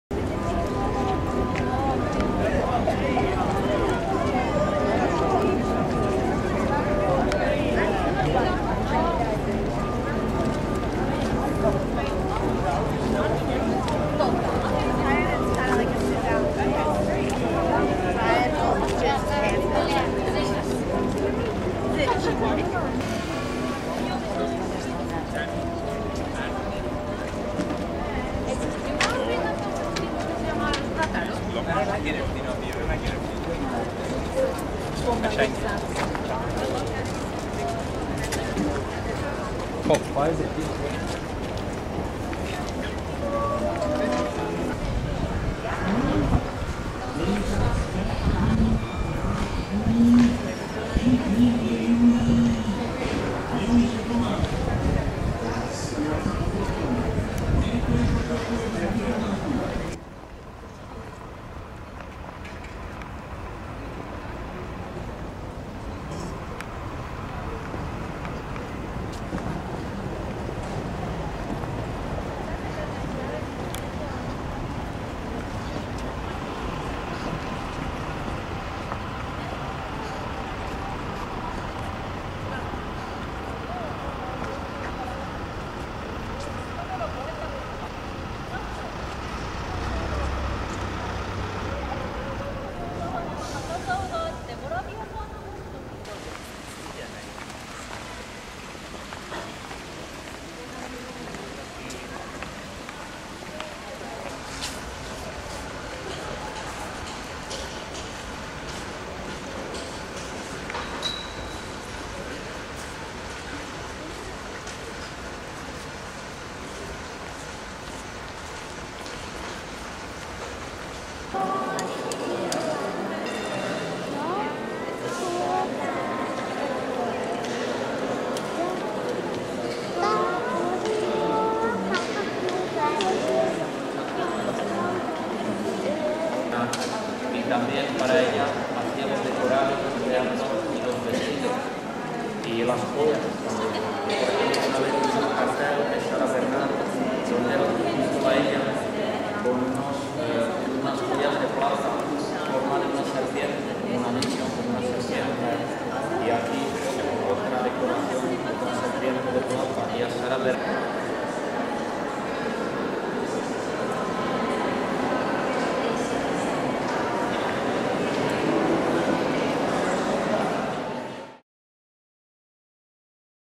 プラハ市内観光１ カレル橋～プラハ城
チェコ プラハ
プラハ城 聖ヴィート教会のステンドグラス綺麗だったよ！